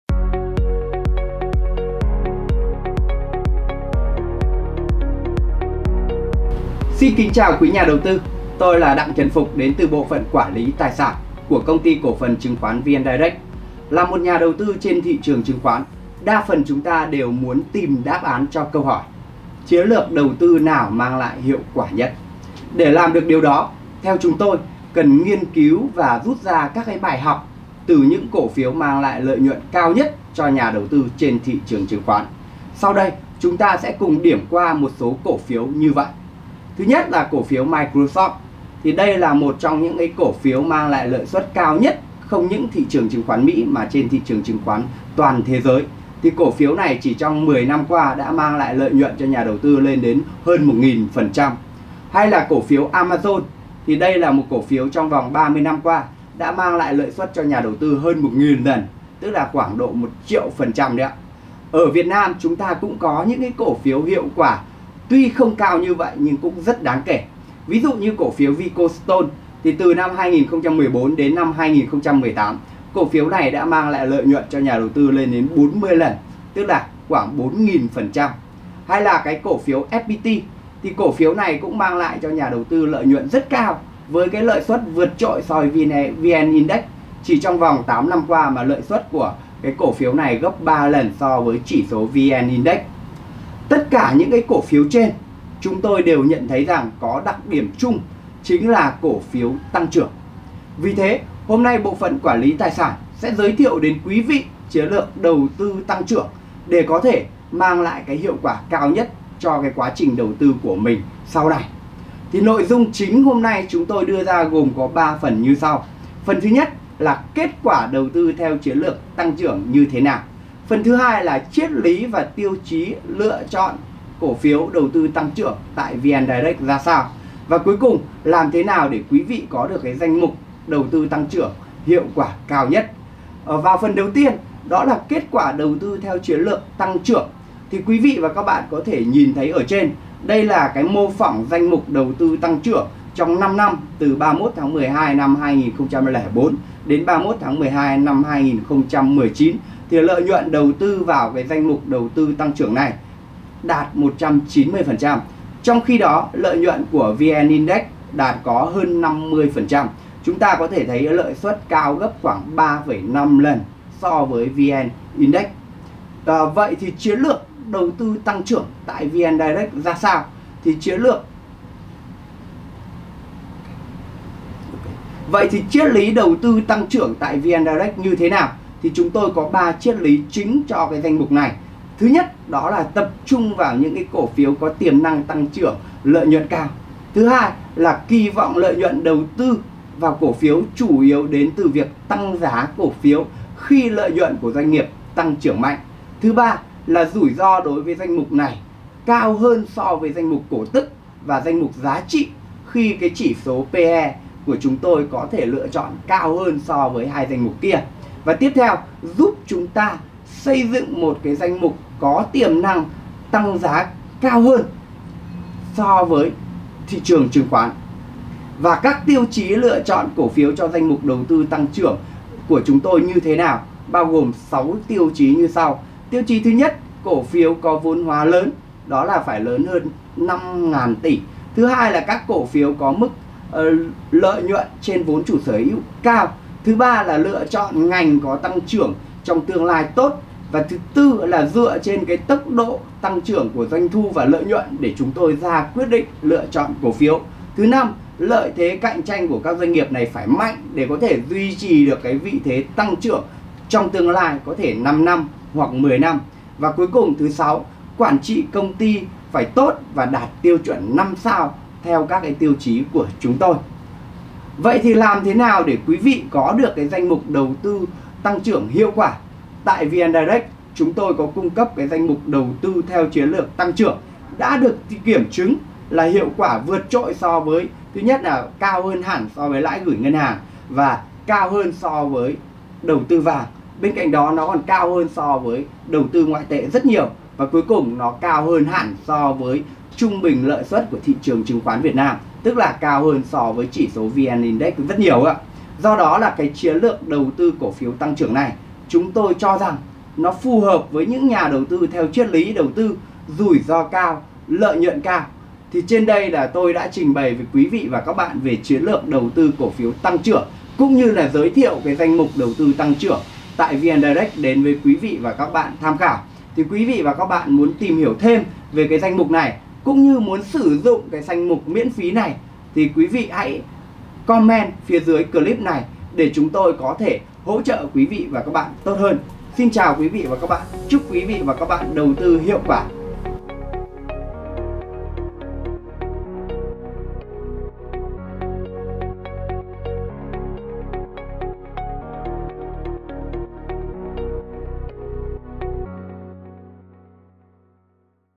Chiến lược đầu tư Tăng trưởng
Trước những giai đoạn bất ổn như tình hình tài chính hiện nay, việc lựa chọn loại cổ phiếu an toàn và hiệu quả để thêm vào danh mục đầu tư là vô cùng khó khăn. Khi đó, cổ phiếu tăng trưởng thường được các nhà đầu tư ưu ái hơn cả bởi nó thuộc về các doanh nghiệp có tốc độ phát triển nhanh hơn so với thị trường chung.
Trong bài chia sẻ này, VNDIRECT sẽ giúp các bạn tìm hiểu về Phương pháp đầu tư cổ phiếu tăng trưởng cũng như cách thức tìm kiếm một cổ phiếu tốt cho chiến lược đầu tư của mình.
Kết quả đầu tư theo chiến lược đầu tư Tăng trưởng như thế nào?
Triết lý và tiêu chí lựa chọn chiến lược đầu tư Tăng trưởng tại VNDIRECT?
Làm thế nào để có danh mục đầu tư Tăng trưởng hiệu quả cao?

Kênh góc nhìn chuyên gia về các vấn đề tâm điểm trên thị trường tài chính chứng khoán:

Kênh thông tin tài sản đầu tư phát sóng 8:00AM mỗi ngày từ thứ 2 đến thứ 6: